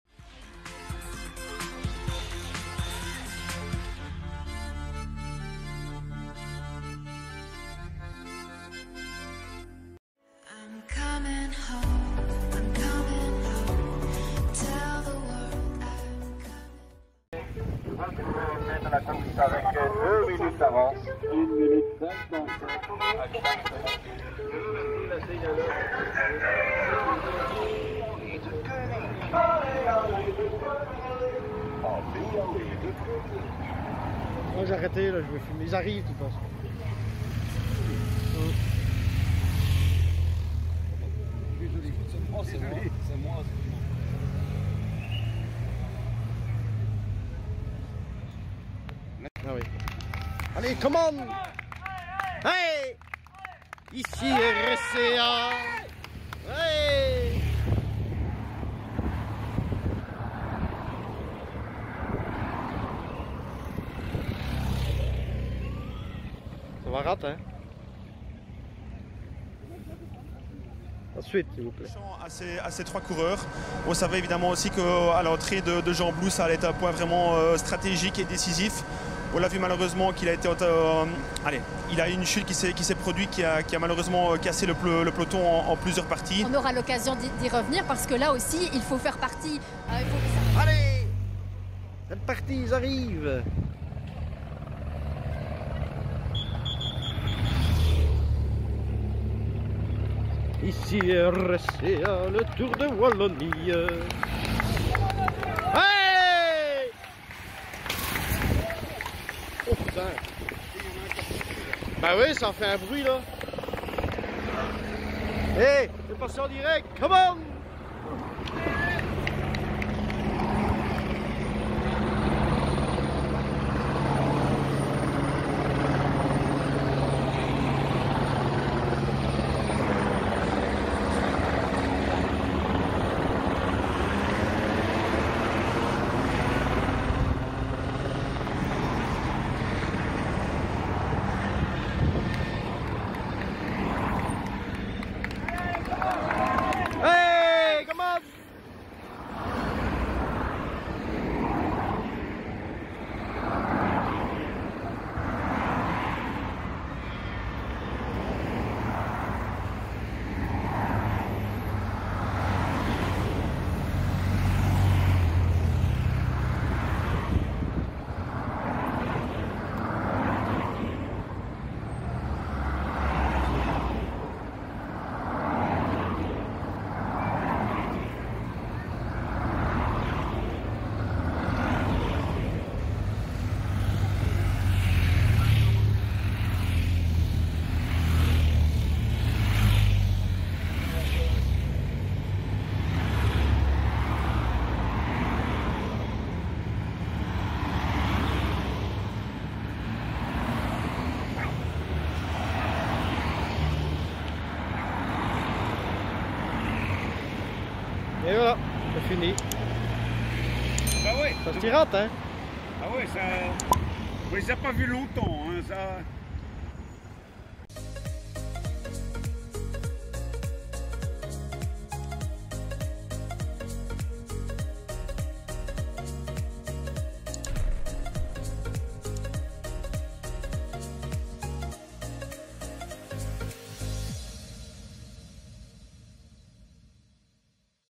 Le Tour de Wallonie 🚲 | au rond-point des 3 clés | à Gembloux [ 24/07/2023 ] 🚲
Le Tour de Wallonie - au rond-point des 3 clés à Gembloux

Le Tour de Wallonie du 22 au 26 juillet 2023

générique : 00:00
intro : 00:10
Le Tour de Wallonie : 00:17
Outro : 03:57